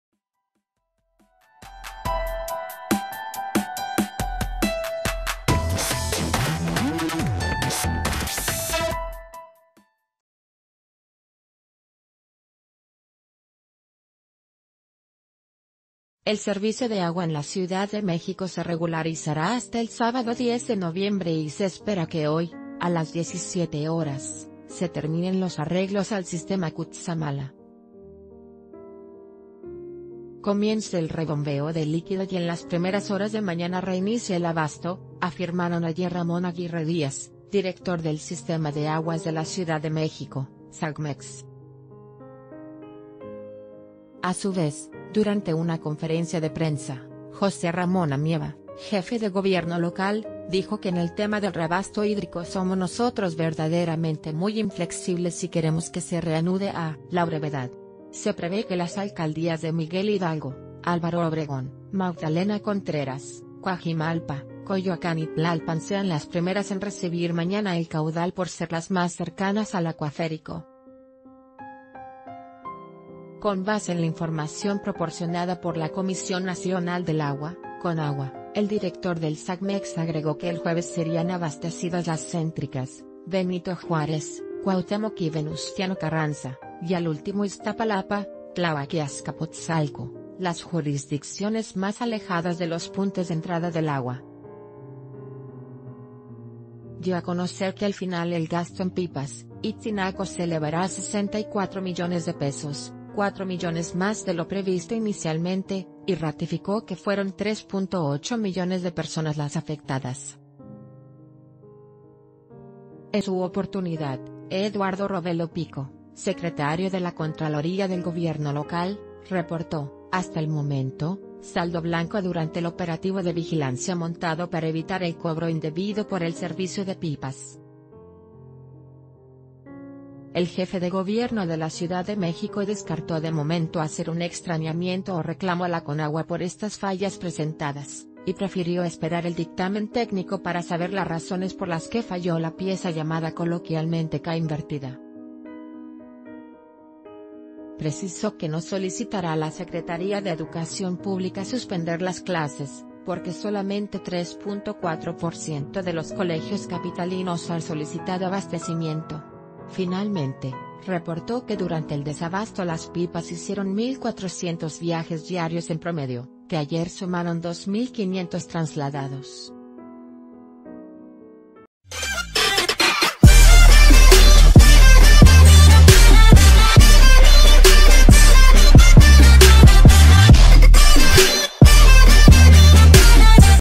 Agua llegará al 100% desde este sábado
El servicio de agua en la Ciudad de México se regularizará hasta el sábado 10 de noviembre y se espera que hoy, a las 17:00 horas, se terminen los arreglos al Sistema Cutzamala, comience el rebombeo de líquido y en las primeras horas de mañana reinicie el abasto, afirmaron ayer Ramón Aguirre Díaz, d...